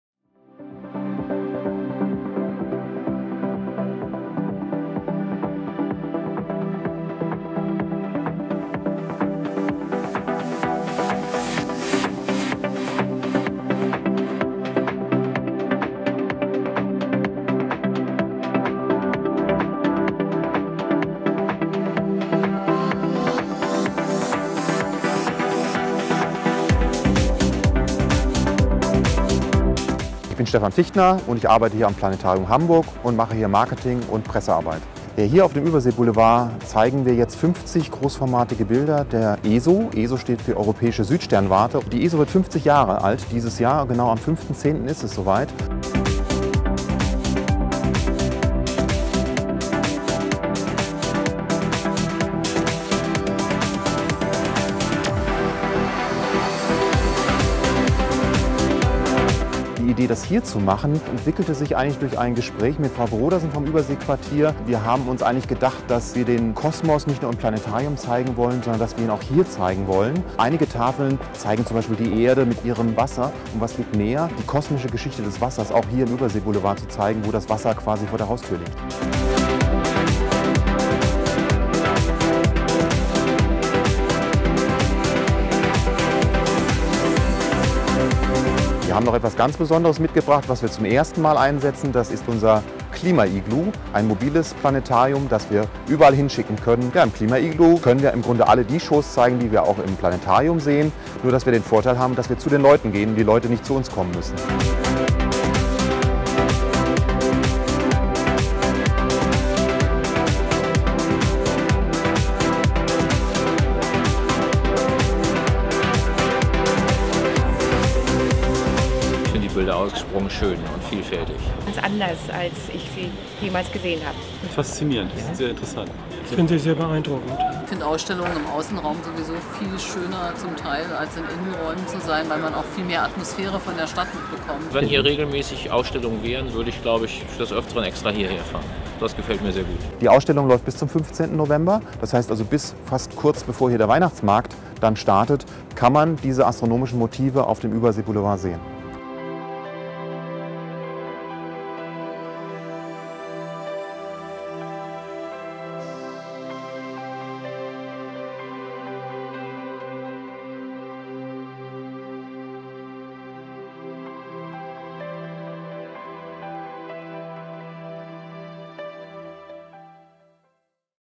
„Faszinierendes Universum"
Die Weiten des Weltraums erleben -- das können die Besucher des Überseequartiers vom 20. September bis zum 15. November im Herzen der HafenCity. Bei der Open-Air-Ausstellung „Faszinierendes Universum -- Der Kosmos durch die Augen der Europäischen Südsternwarte" zeigt das Überseequartier in Zusammenarbeit mit dem Planetarium Hamburg die schönsten Fotografien aus den letzten 50 Jahren Europäische Südsternwarte (ESO). Die insgesamt 50 Bilder im Format 1,20 mal 1,80 Meter zeigen das Universum, die Milchstraße und unseren blauen Planeten -- die Erde. Mit hochmodernen Teleskopen der ESO aufgenommen, ermöglichen sie den Besuchern einen faszinierenden Einblick ins All.